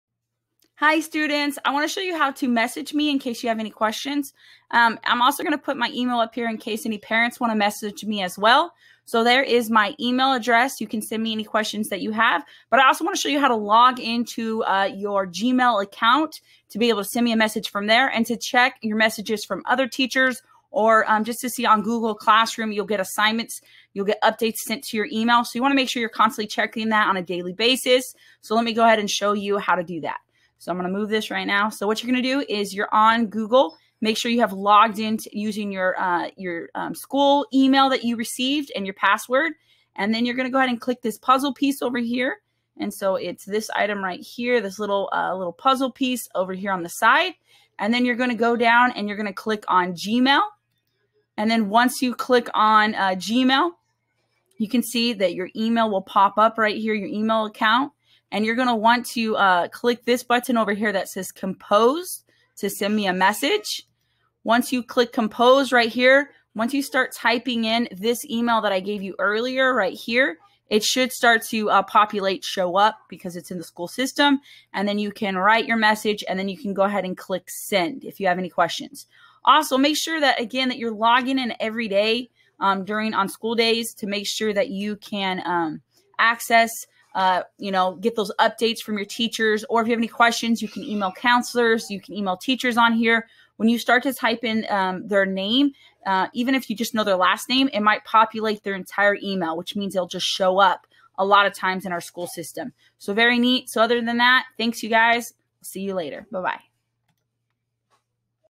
How to Check Your School's Email on GMAIL
Quick tutorial I made to show my students how to check their school's email on GMAIL and how to message me with any questions.